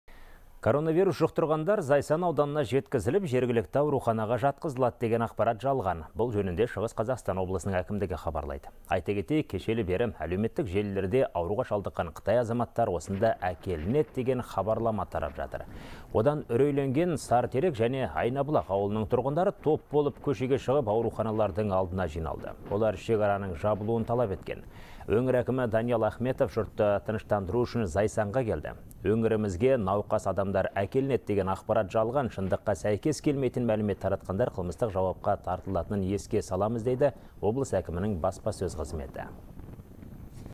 Зайсанға коронавирус жұқтырғандар келетіні туралы ақпарат жалған болып шықты
Коронавирус жұқтырғандар Зайсан ауданына жеткізіліп, жергілікті ауруханаға жатқызылады деген ақпарат жалған. Бұл жөнінде Шығыс Қазақстан облысының әкімдігі хабарлады.